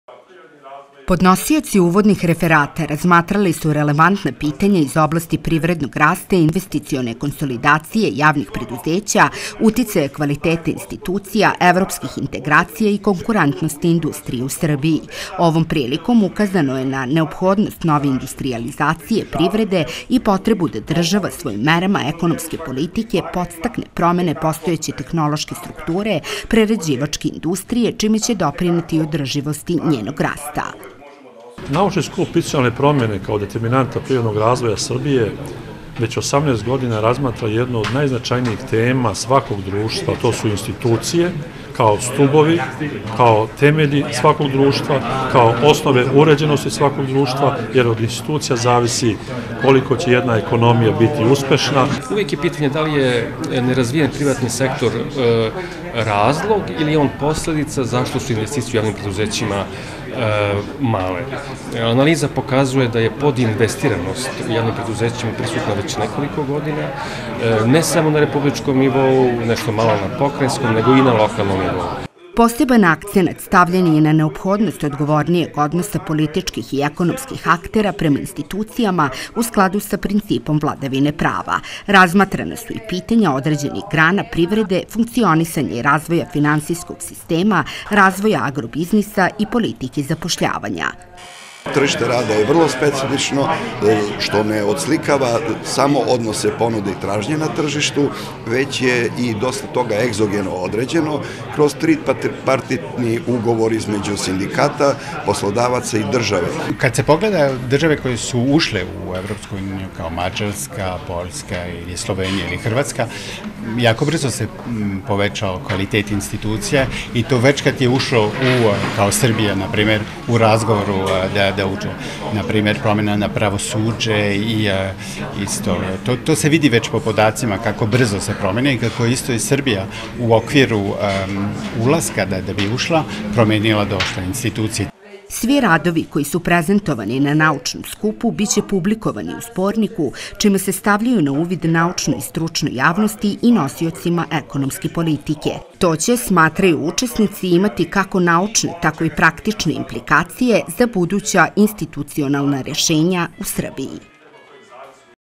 Naučni skup ekonomista u Kragujevcu
Како је привредни раст повезан са инвестиционом консолидацијом јавних предузећа, колико је српска индустрија конкурентна и како институционална решења могу бити у функцији равномерног регионалног развоја биле су теме 18. по реду научног скупа који је одржан на Економском факултету Универзитета у Крагујевцу.